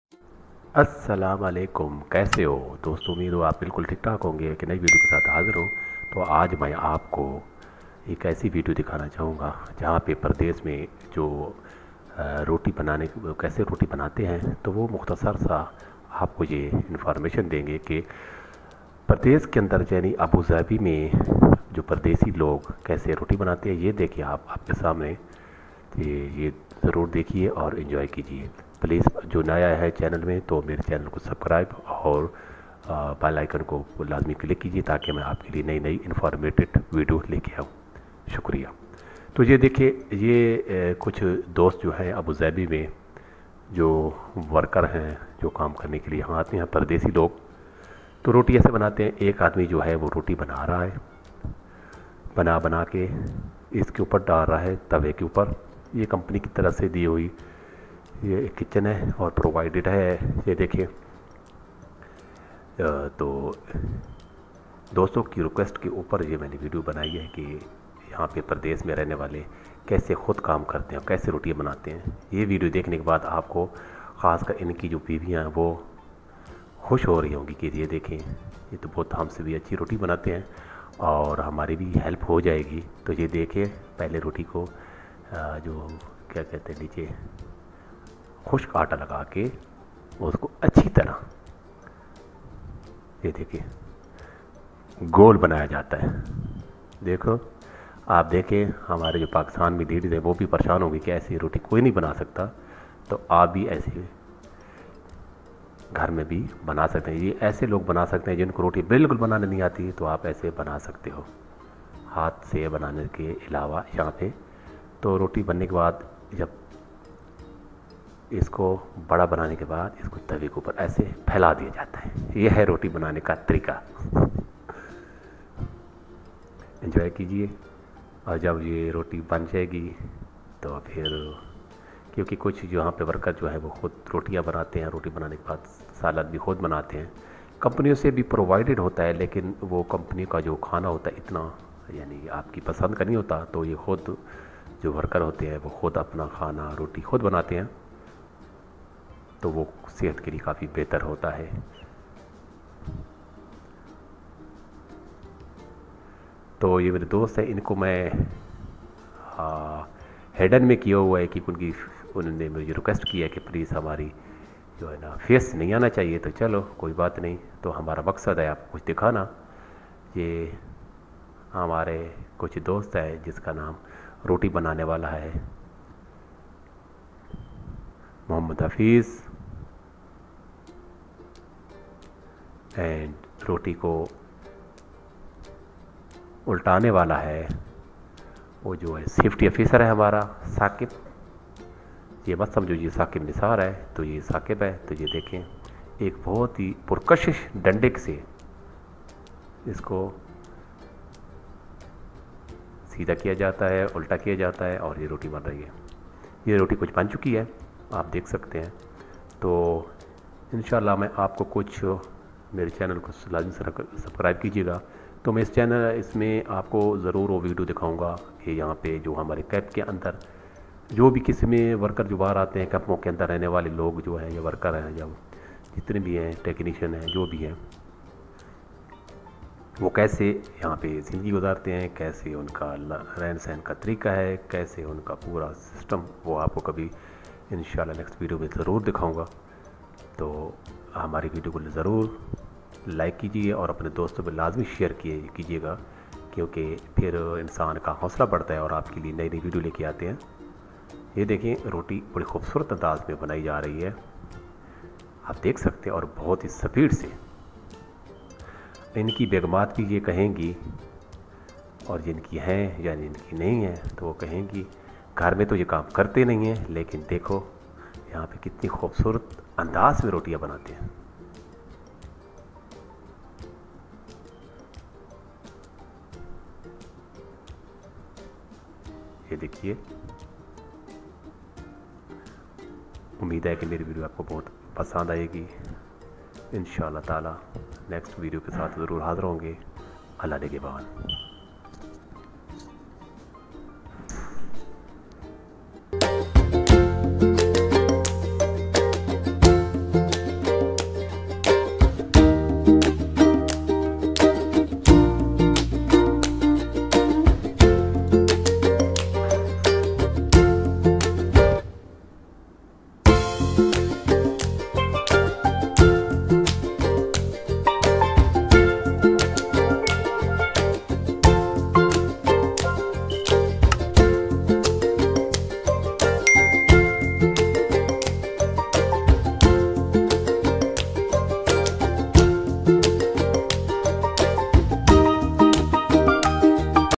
Overseas Pakistan life in Abu Dhabi UAE|| Make breads in new method of Pakistani worker techniq
Overseas Pakistani life in Abu Dhabi UAE
NEW method of making bread in Abu Dhabi UAE
Pakistani Ka Abu Dhabi main roti banane Ka nea Andaz